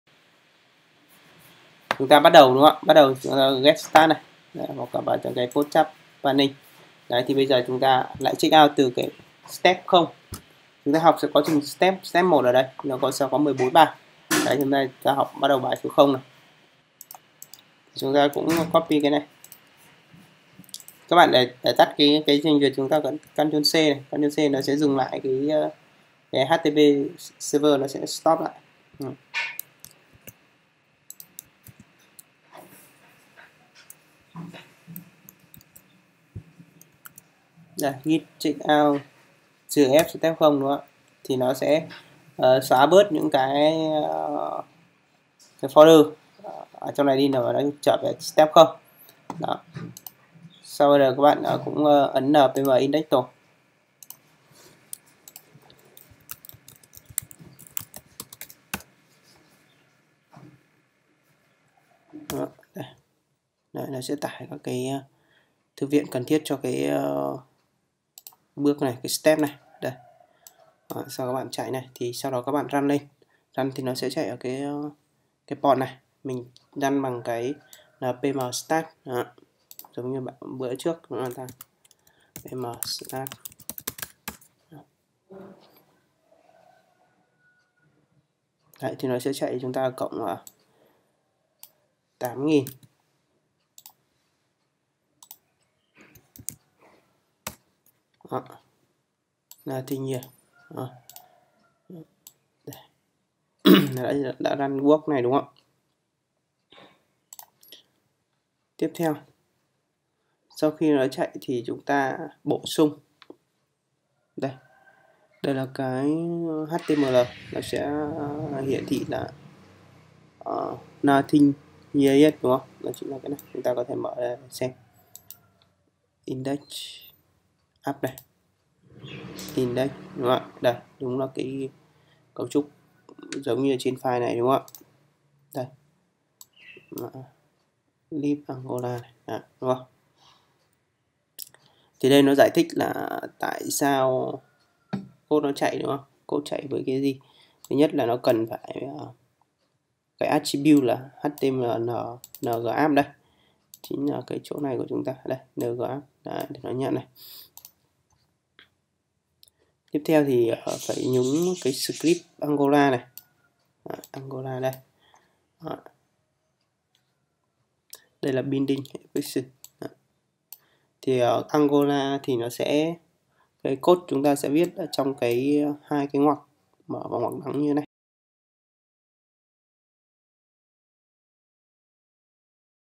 Học angular qua ví dụ phonecat bài 1|dandev
Tiếp tục series học angular qua ví dụ phonecat. Ở đây mình nói tới là angularjs version 1. Theo mình thì các bạn mới tiếp cận angularjs thì học verison 1 sẽ dễ tiếp cận hơn. Sau khi hoàn thành 14 bài học angular qua ví dụ phonecat bạn sẽ biết cách sử dụng angularjs cho việc tạp template hay sử dụng $http để lấy data qua api.